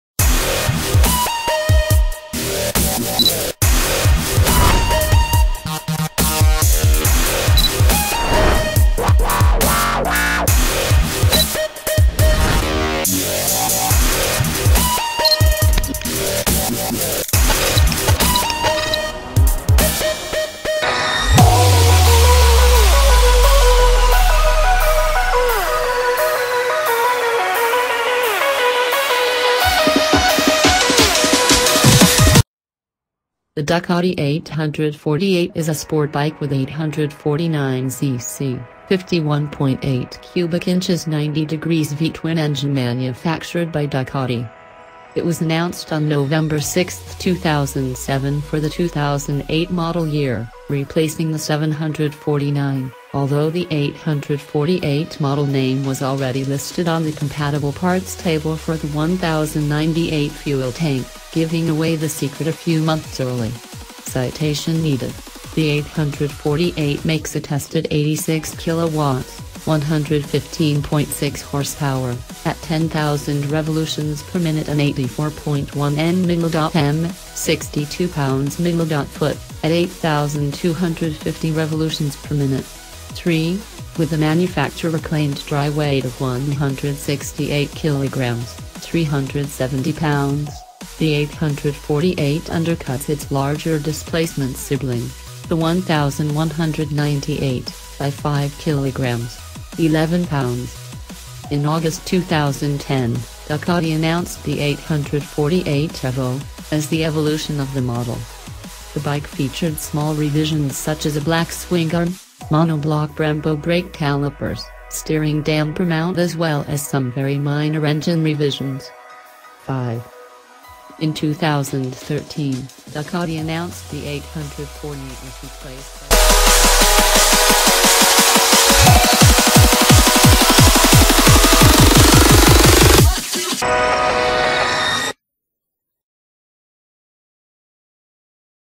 WOW AMAZING Ducati 848 Evo Review
WOW AMAZING Ducati 848 Evo Review - The Ducati 848 is a sport bike with a 849 cc (51.8 cu in) 90° V-twin engine manufactured by Ducati. It was announced on November 6, 2007 for the 2008 model year, replacing the 749, although the 848 model name was already listed on the compatible parts table for the 1098 fuel tank, giving away the secret a few months early.[citation needed] The 848 makes a tested 86 kW (115.6 hp) at 10,000 rpm and 84.1 N·m (62 lb·ft) at 8,250 rpm.[3] With a manufacturer claimed dry weight of 168 kg (370 lb), the 848 undercuts its larger displacement sibling, the 1198, by 5 kg (11 lb).

In August 2010, Ducati announced the 848 Evo, as the evolution of the model. The bike featured small revisions such as a black swingarm, Mono-block Brembo brake calipers, steering damper mount as well as some very minor engine revisions.[5]

 $13,995